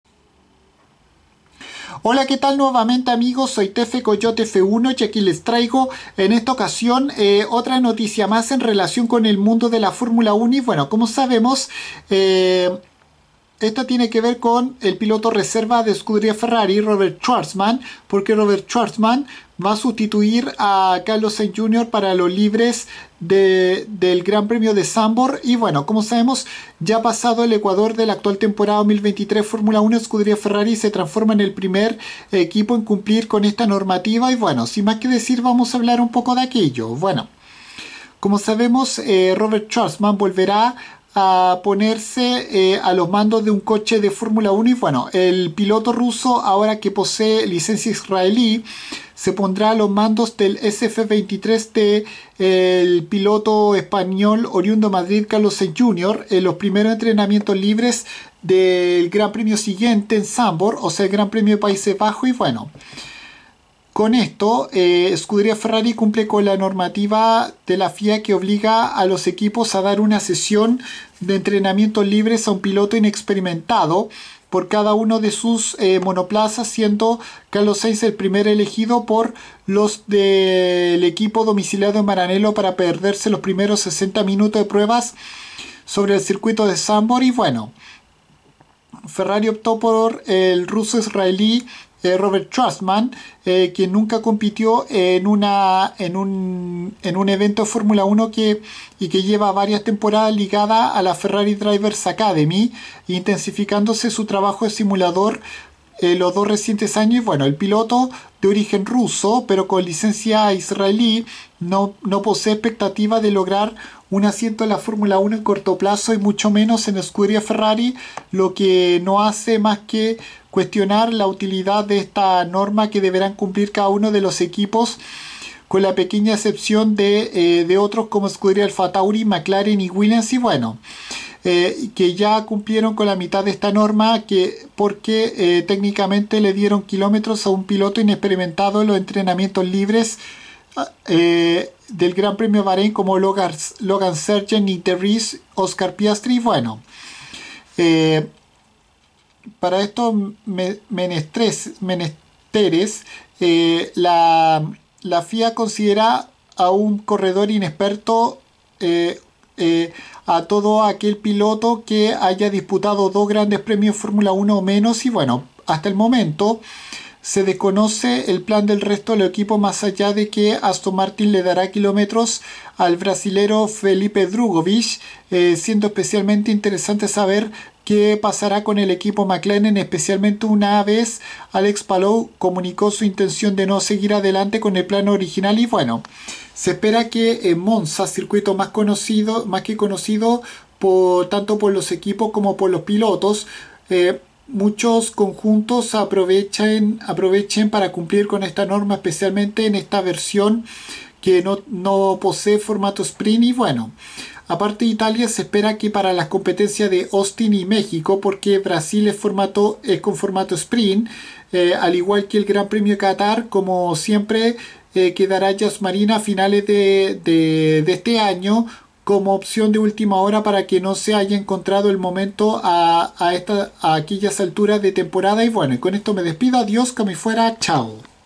Robert Shwartzman sustituye a Carlos Sainz Jr. para los FP1 de Zandvoort
Para esta nueva versión 2023 del Gran Premio de Países Bajos(u Holanda) el piloto ruso-israelí tomará el lugar del piloto español oriundo de Madrid en Scuderia Ferrari